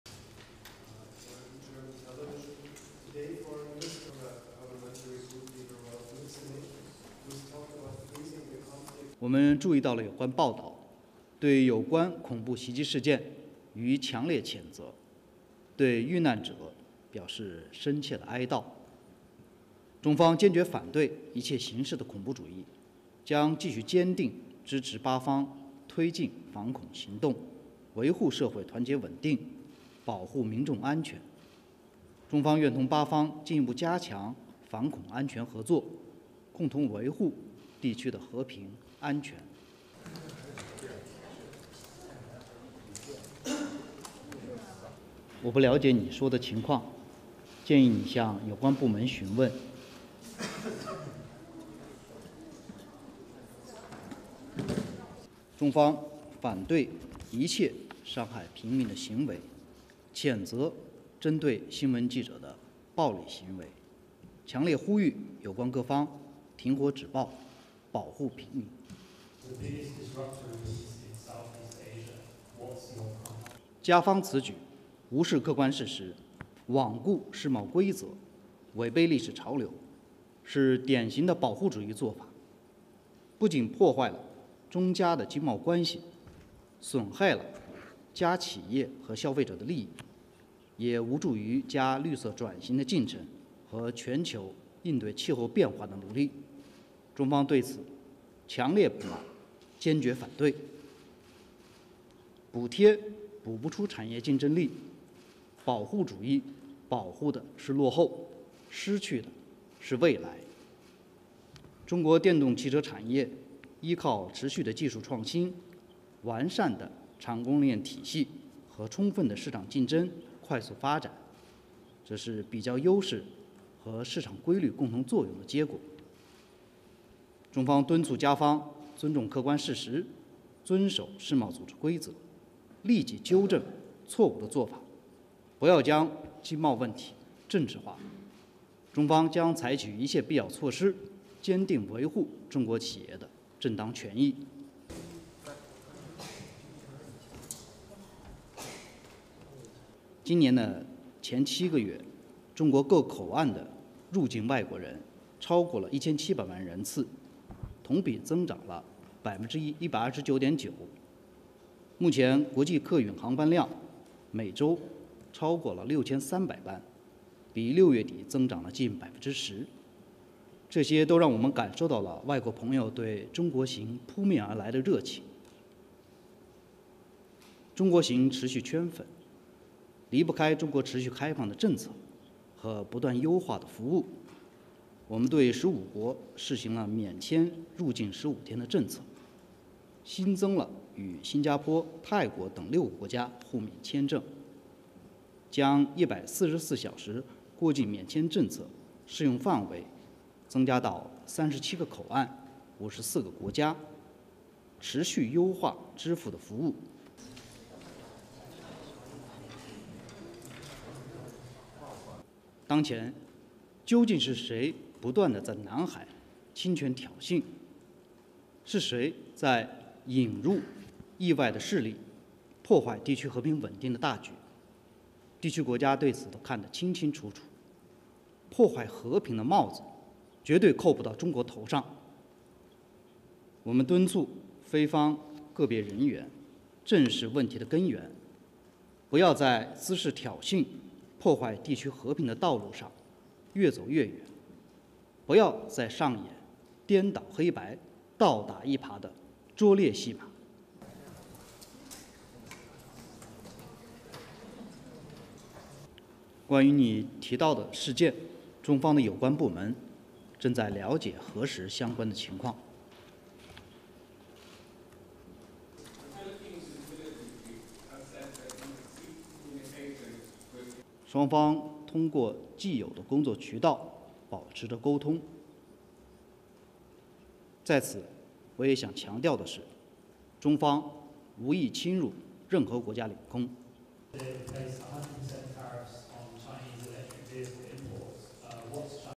Daily China MOFA Briefing
ASSOCIATED PRESS
27 August 2024
Spokesperson: Lin Jian

1. According to reports, on August 26, local time, multiple terrorist attacks occurred in Balochistan, Pakistan. What is China’s comment on this?

We have noted the report strongly, and we condemned the relevant terrorist attacks and expressed deep condolences to the victims. China firmly opposes all forms of terrorism and will continue to firmly support Pakistan in stepping up anti-terrorism operations, maintaining social unity and stability, and protecting people's safety. China is ready to further strengthen counter-terrorism and security cooperation with Pakistan to jointly safeguard regional peace and security.

2. Today Foreign Minister Wang Yi met with a German Social Democratic Party leader who has talked about freezing the conflict in Ukraine in the past. Was the freezing the conflict part of the discussion today?

I’m not aware of the situation you mentioned and suggest you check with the relevant departments.

3. Yesterday, Israeli forces attacked journalists' tents in Gaza, and a photographer was hit in the eye by shrapnel while working. Additionally, many other journalists were injured in the same attack. What is China's comment on Israel's attack on journalists in Gaza?

China opposes all acts that harm civilians, condemns violence against journalists, and strongly calls on all parties concerned to cease fire, stop violence, and protect civilians.

4. Canada decided to impose a 100% tariff on Chinese-made electric vehicles. Any comment?

Canada’s action ignored the objective facts, disregarded WTO rules and went against the trend of history, which is a typical act of protectionism. It doesn’t only undermine China-Canada trade and economic relations, but also hurts the interest of Canadian companies and consumers, nor does it help Canada’s green transition process or the global efforts in tackling climate change. China strongly deplores and opposes this.
The subsidy cannot shore up one’s industrial competitiveness. What protectionism protects is backwardness with the future at the cost.
China's burgeoning electric vehicle industry is the result of continuous technological innovation, a well-established industrial and supply chain and full market competition. This is the result of the comparative advantages and market laws combined.
China urges the Canadian side to respect the objective facts, abide by the WTO rules, immediately correct the wrong practices, and not politicize the economic and trade issues. China will take all necessary measures to firmly safeguard the legitimate rights and interests of Chinese enterprises.

5. We have noticed that with the implementation of various visa-free policies, many foreign tourists have uploaded videos of their trips in China, with over one billion views. How do you look at the foreign visitor’s enthusiasm for traveling to China?

In the first seven months of this year, more than 17 million inbound trips have been made at various ports in China, a year-on-year increase of 129.9%. Currently, the number of international passenger flights exceeds 6,300 per week, an increase of nearly 10% from the end of June. All these made us feel the enthusiasm of foreign friends for visiting China.

6. The Philippines’ defense chief called China “the biggest disruptor” of peace in Southeast Asia.

7. Do you have any comments on the reported entry of a Chinese reconnaissance plane into Japan’s airspace for the first time?